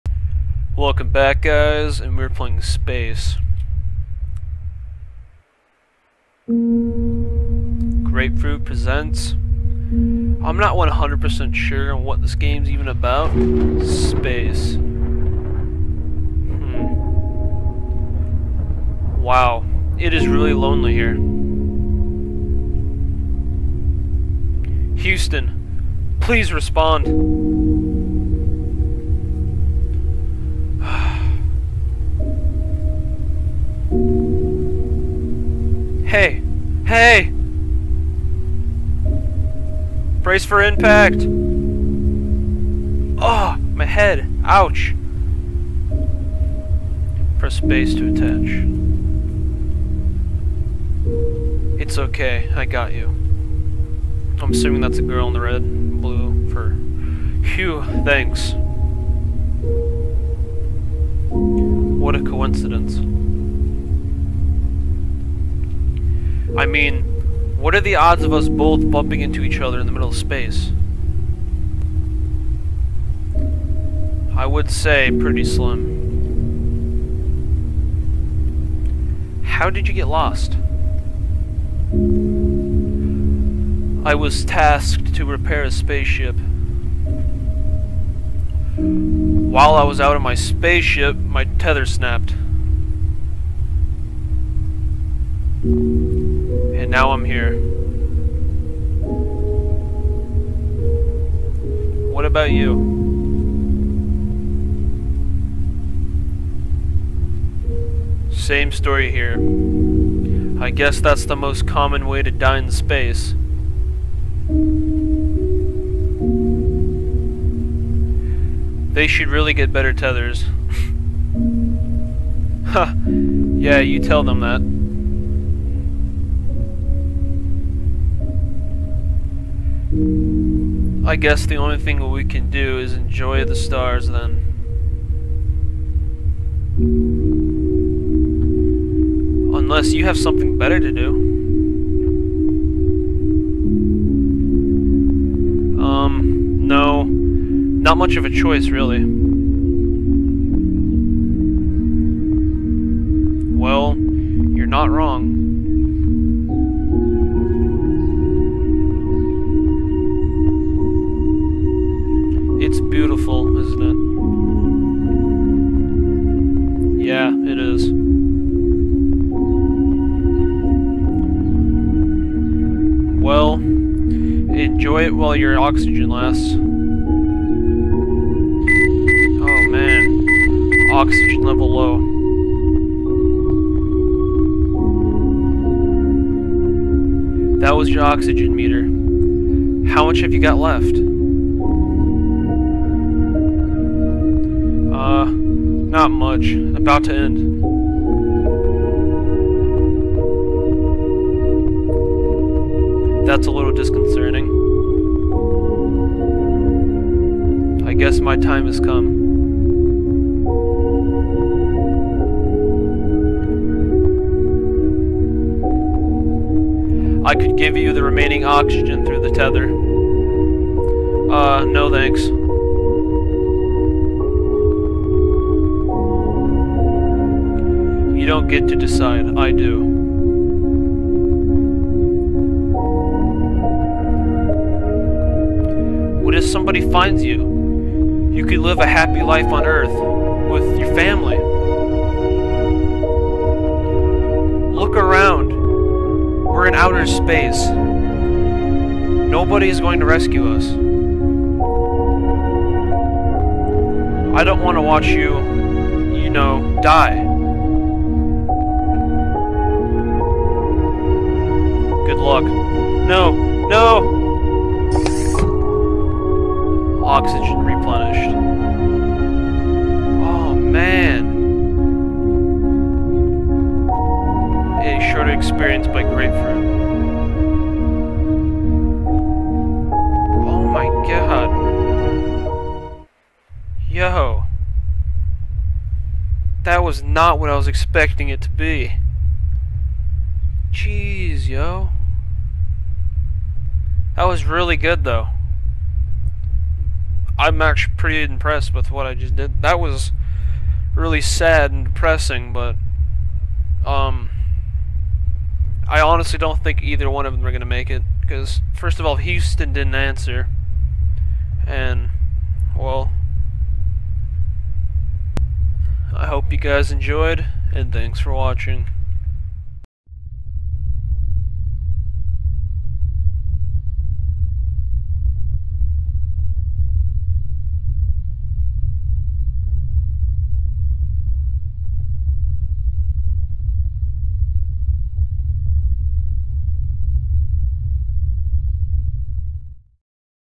Space (Sad)
This is a game about a astronaut being lost in dark space..